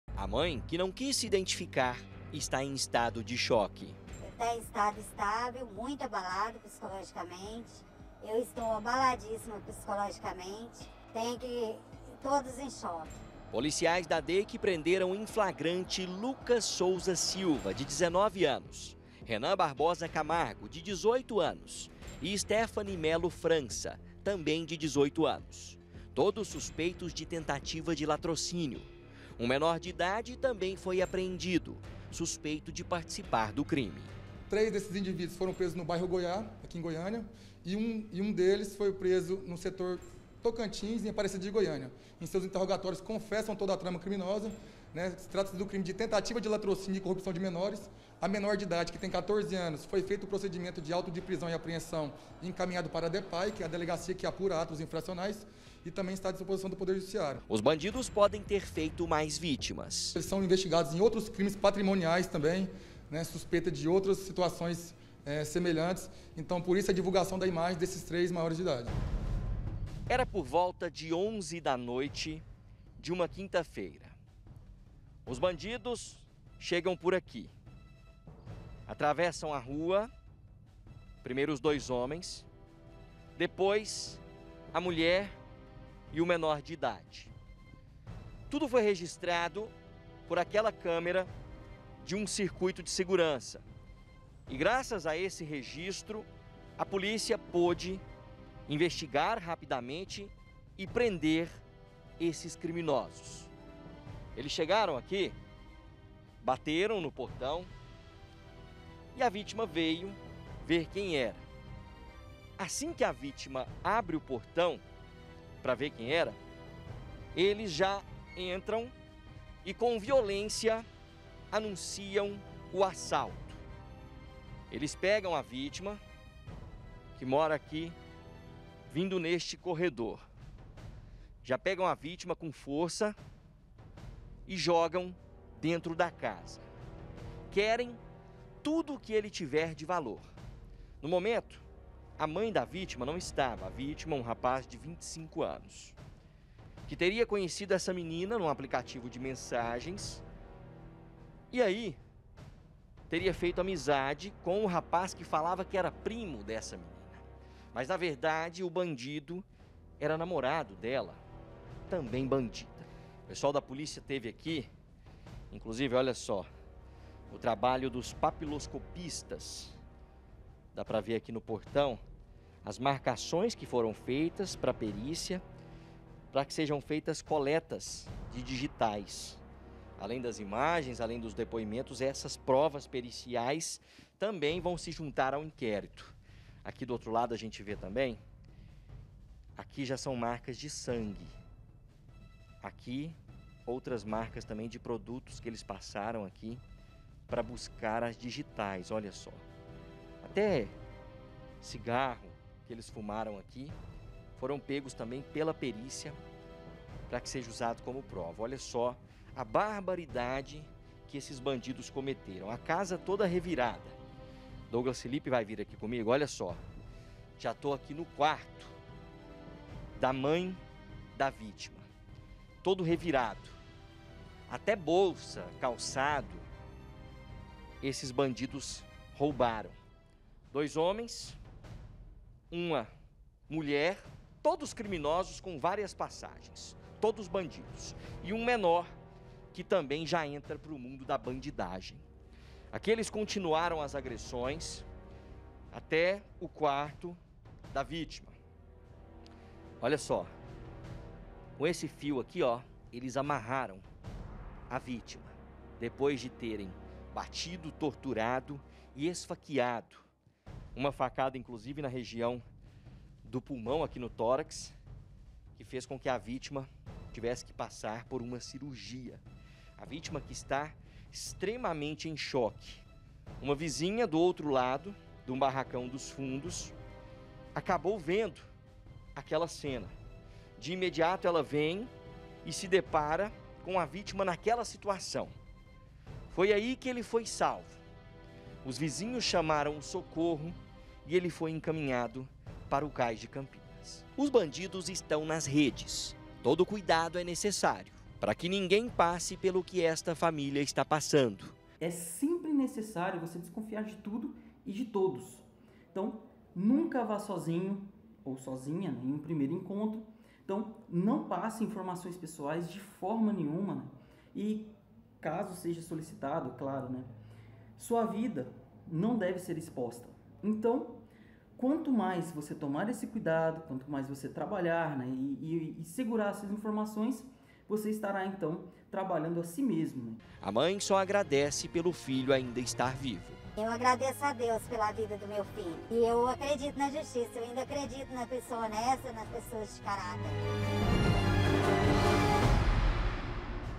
CONTINUA INTERNADO: HOMEM É TORTURADO POR BANDIDOS QUE CONHECEU EM APLICATIVO
HOJE EU ESTIVE COM EXCLUSIVIDADE NA CASA DO JOVEM QUE FOI VÍTIMA DE UMA TENTATIVA DE LATROCÍNIO. ELE FOI AGREDIDO, TORTURADO, E NO FINAL ESFAQUEADO. ELE CAIU NO GOLPE PORQUE CONHECEU UMA MENINA NUM APLICATIVO DE RELACIONAMENTO CRISTÃO. A MOÇA O ATRAIU PARA A EMBOSCADA.